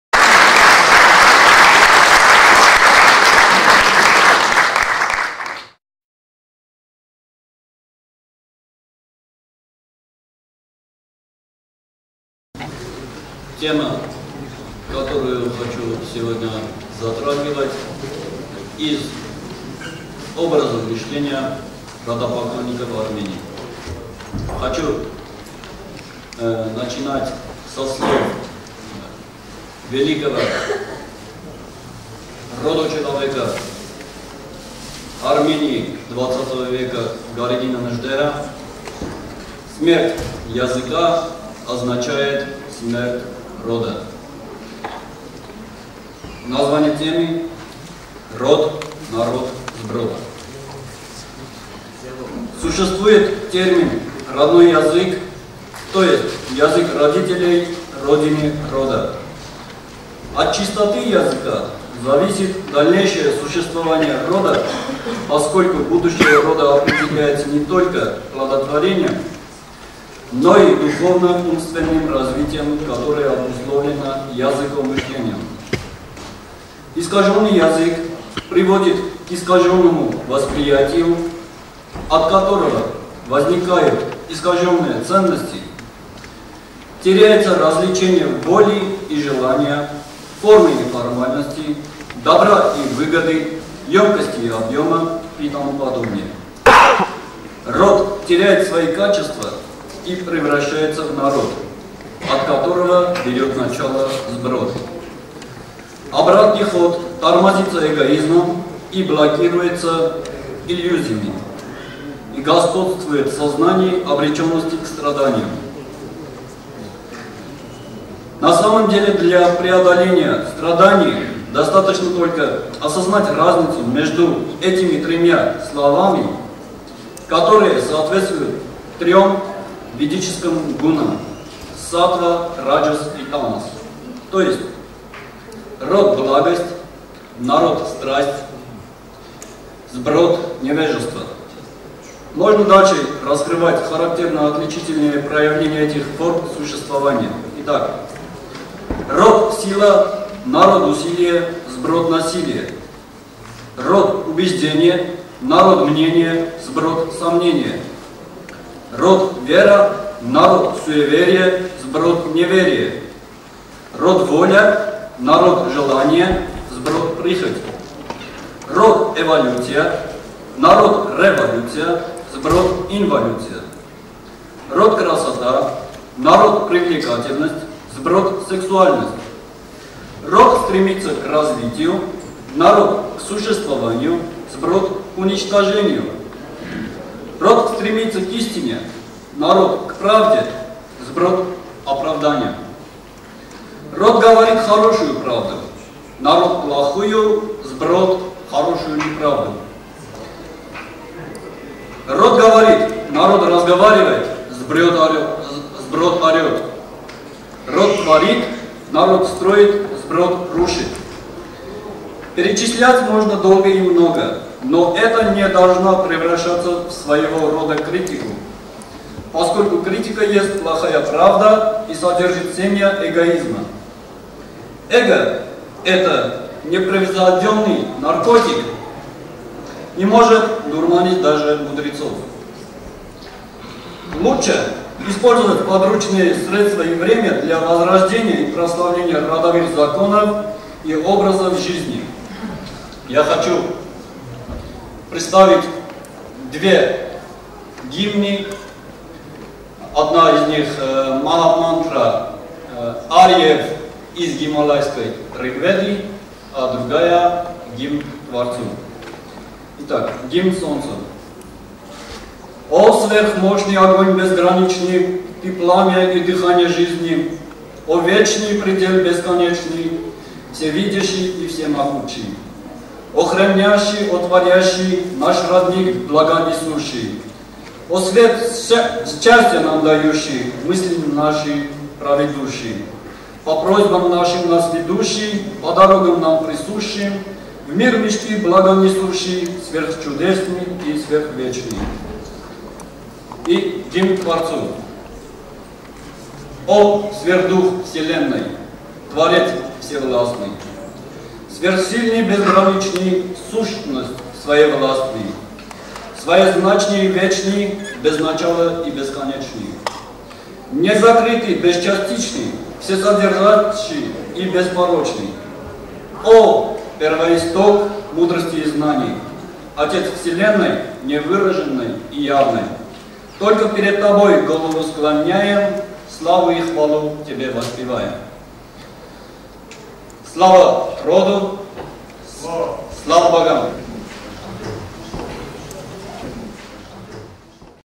Андроник Атоян. Род Народ Сброд.avi
Существует термин Родной язык, то есть язык родителей, Родины Рода. От чистоты языка зависит дальнейшее существование Рода, поскольку будущее Рода объединяется не только плодотвореньем, но и духовно-умственным развитием, которое обусловлено языком мышления.